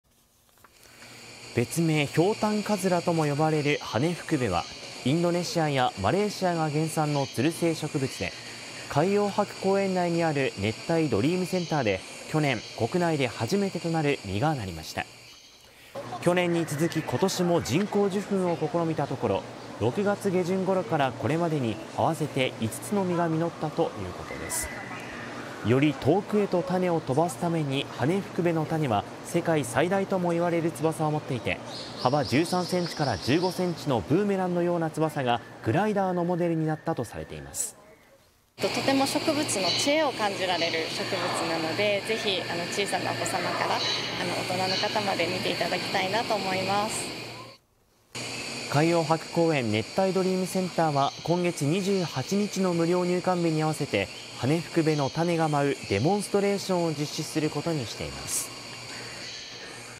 グライダーのモデルとも言われる“翼を持つ種子”ハネフクベ 熱帯ドリームセンターで2年連続実る
翼の付いた種子がグライダーのモデルになったとされる、東南アジア原産のつる性植物『ハネフクベ』が海洋博公園の熱帯ドリームセンターで実を付けています。
2023/10/19